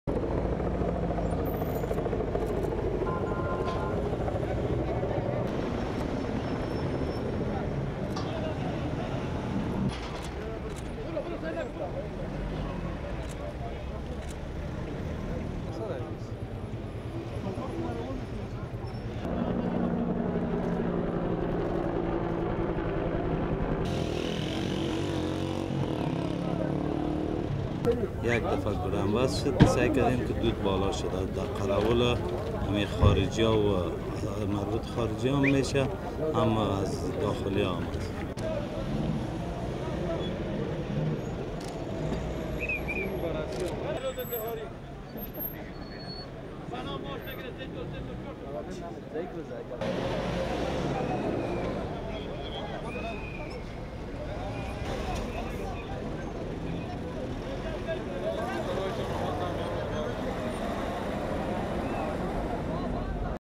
Suicide bomber kills 4 foreign guards in Kabul
(22 Jul 2014) Afghan officials say a suicide bomber on a motorcycle targeted a police compound in Kabul on Tuesday, killing four foreign security guards and wounding six.
Deputy interior minister, General Mohammad Ayub Salangi, said the attacker targeted the compound that houses foreign advisers to the Afghan government as well as the office of the deputy minister for counter-narcotics.
A police spokesman said the attacker managed to reach the entrance gate of the compound and detonated his motorbike packed with explosives on Tuesday morning.
The nationality of the killed guards was not immediately disclosed.
Taliban spokesman Zabihullah Mujahid claimed responsibility for the attack in a statement sent to media.
The Taliban frequently use roadside bombs and suicide attacks against Afghan and NATO forces and government offices in the country.